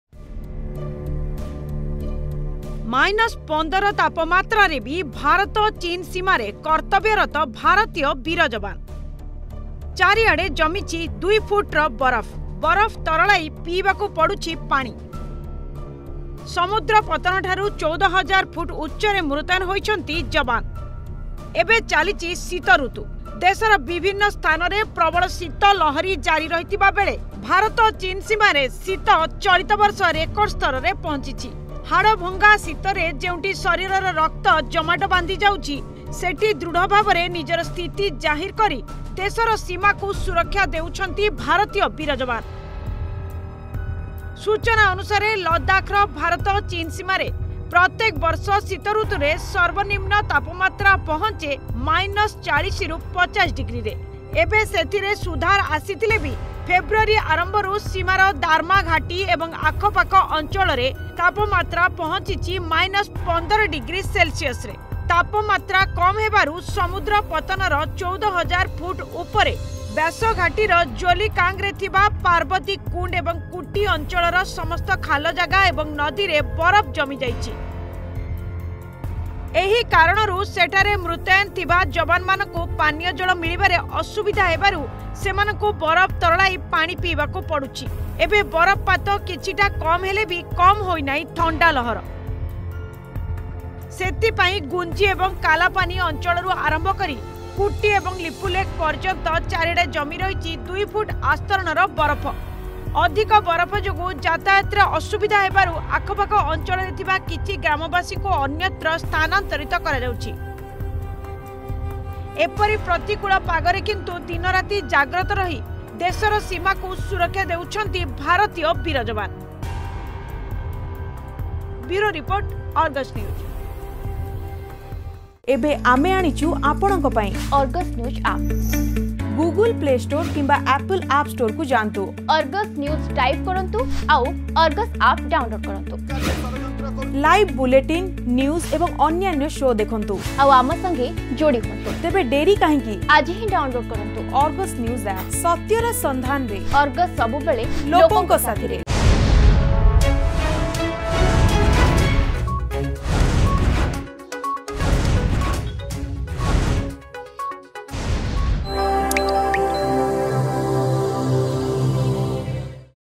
ବରଫ ତରଳେଇ ପାଣି ପିଉଛନ୍ତି ଭାରତୀୟ ଯବାନ
ମାଇନସ୍ ୧୫ ତାମାତ୍ରାରେ ବି ଭାରତ-ଚୀନ୍ ସୀମାରେ କର୍ତବ୍ୟରତ ଭାରତୀୟ ବୀର ଜବାନ୍ । ଚାରିଆଡ଼େ ଜମିଛି ୨ ଫୁଟର ବରଫ୍ । ସେହି ବରଫ ତରଳେଇ ପିଇବାକୁ ପଡ଼ୁଛି ପାଣି । ସମୁଦ୍ର ପତନଠାରୁ ୧୪ ରୁ ୧୮ ହଜାର ଫୁଟ ଉଚ୍ଚରେ ମୁତୟନ ହୋଇଛନ୍ତି ଜବାନ୍.